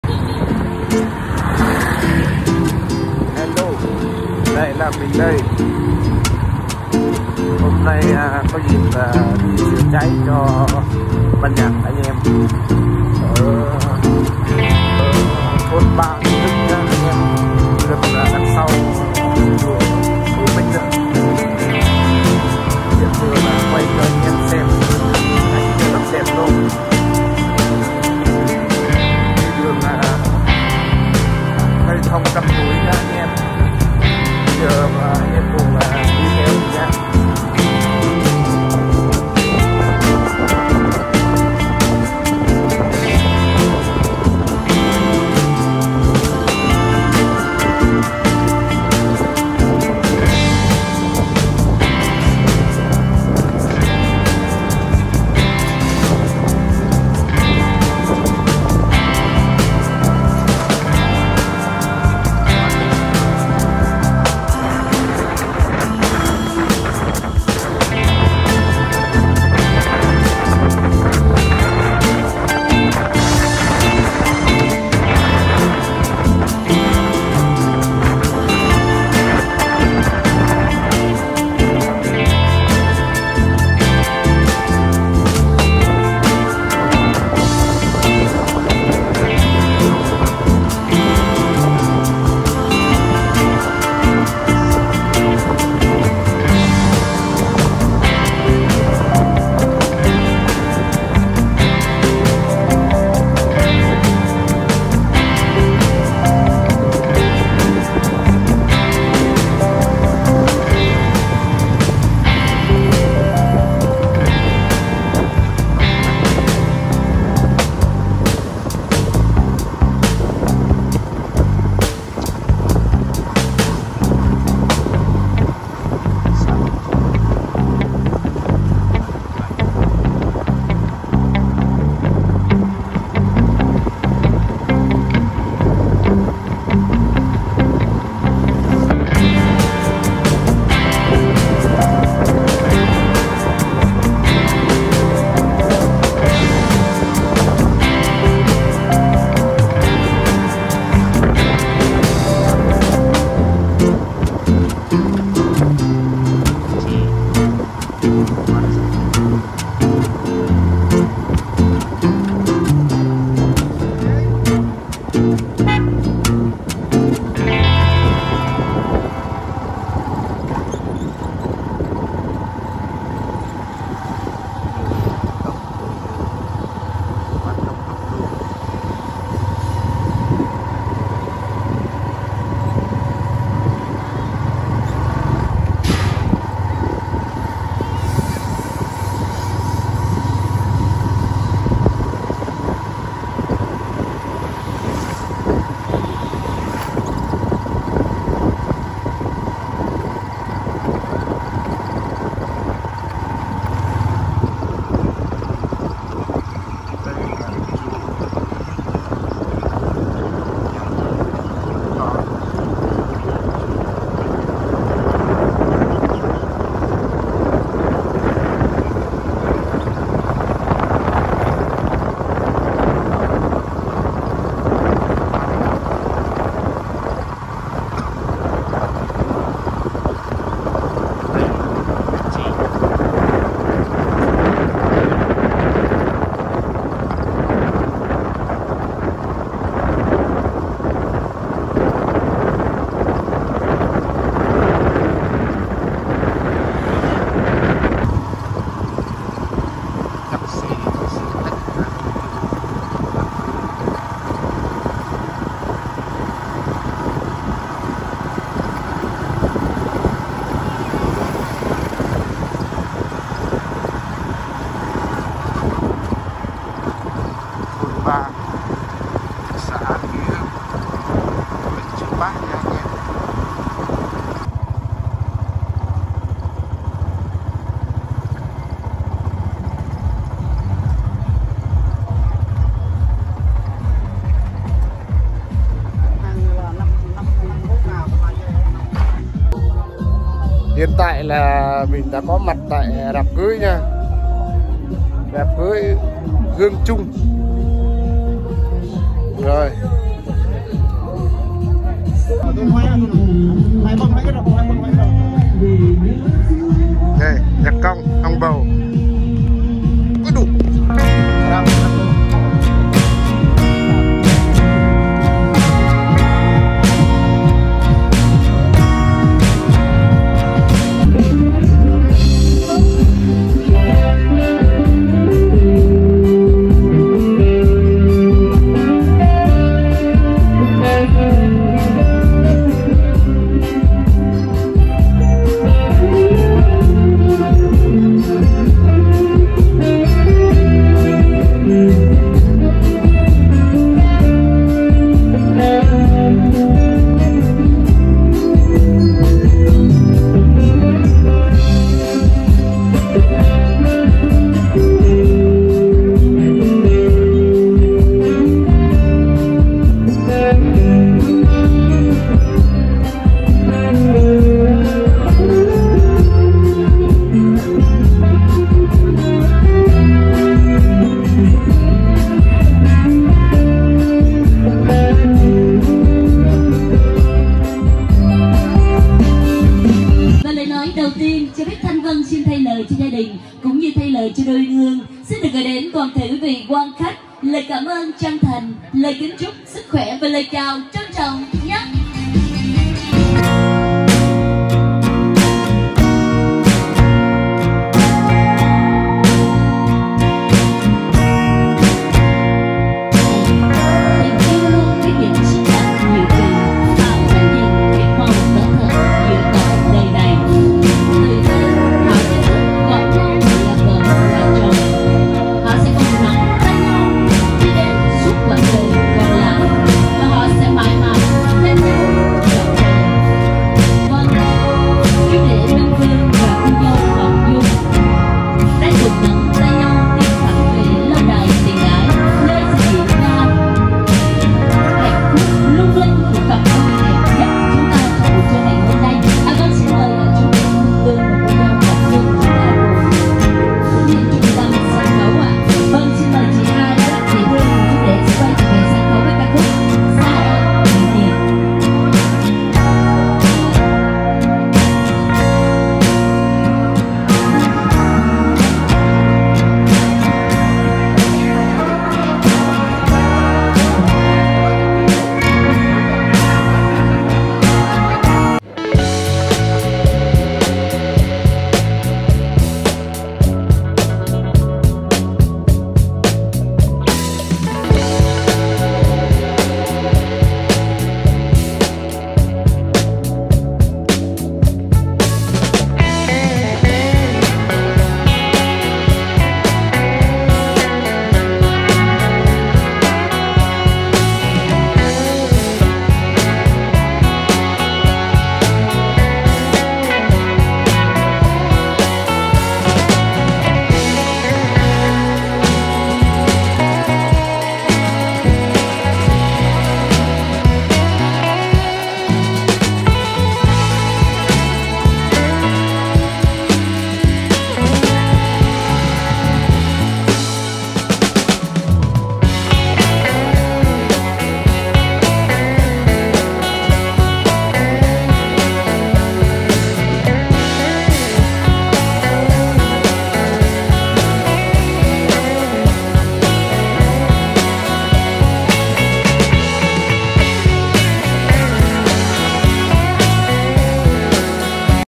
Đi Chữa Cháy Cho Ban Nhạc Yu Đa Ỡ Xã Nghĩa Hưng Chư Pah
Đi làm nhạc đám cưới